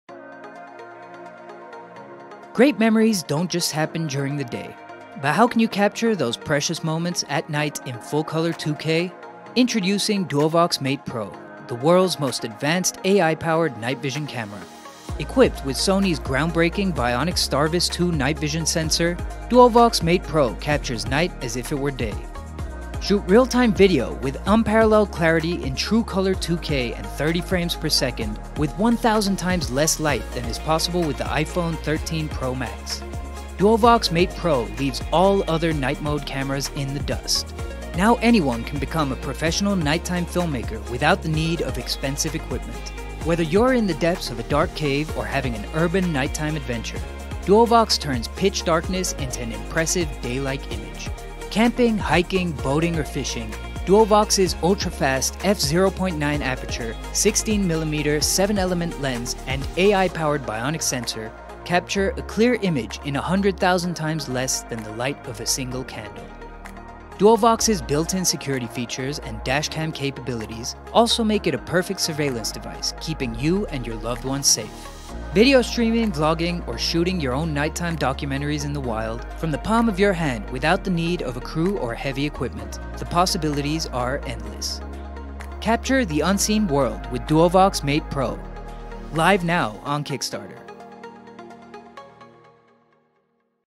Now on Kickstarter: Duovox Mate Pro: Observe The Night In Low-Light, Full-Color
Duovox Mate Pro: Observe the Night in Low-Light, Full-Color
Turn Night Into Day With The Most Advanced True-Color Night Vision Camera for Your Next Outdoor, Boating, Travel, or City Adventures.